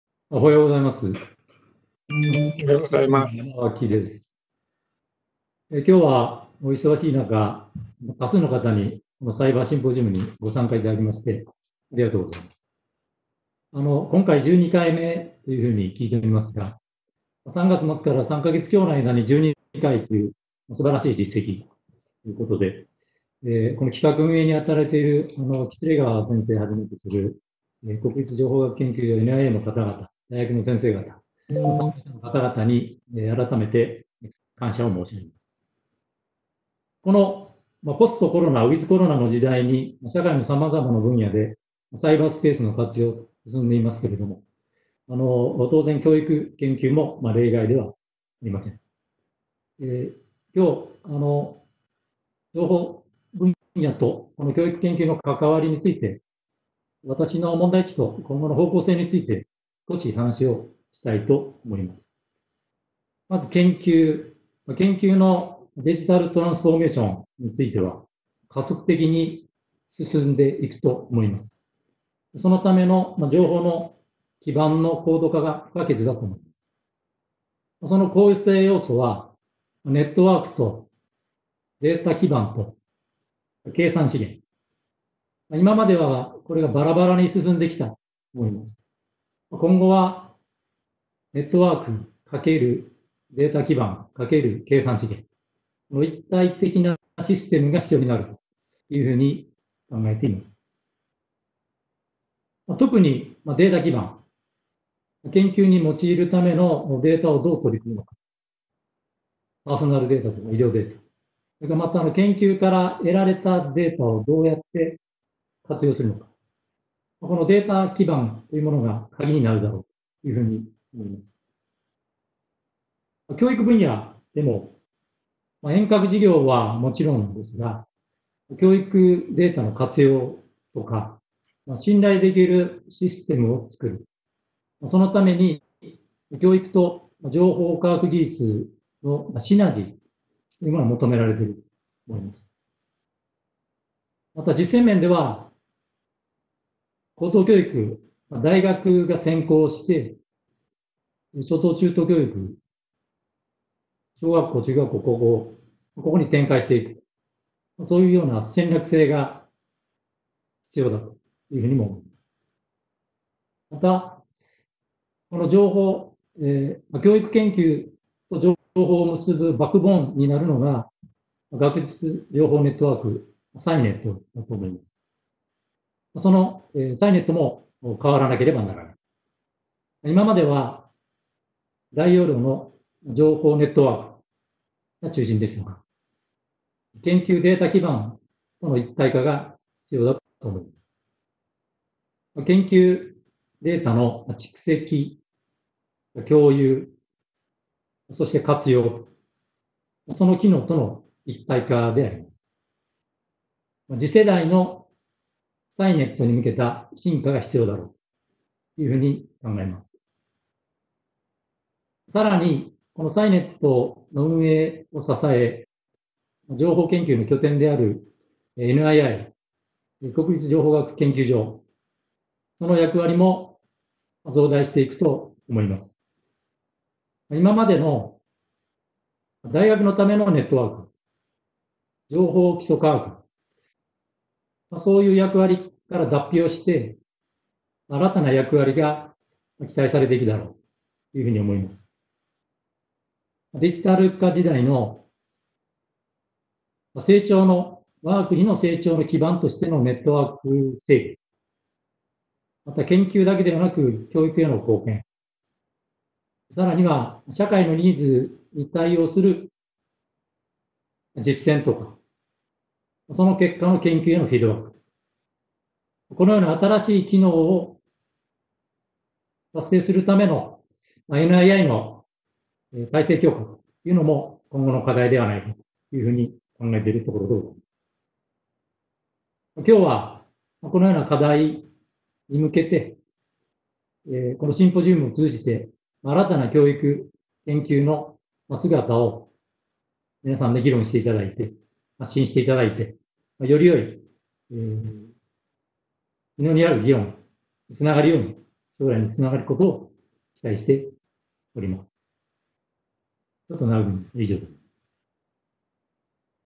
「文部科学省ご挨拶」山脇 良雄 文部科学審議官
「文部科学省ご挨拶」
山脇 良雄 文部科学審議官
【第12回】４月からの大学等遠隔授業に関する取組状況共有サイバーシンポジウム
2020年7月10日オンライン開催
主催　国立情報学研究所　大学の情報環境のあり方検討会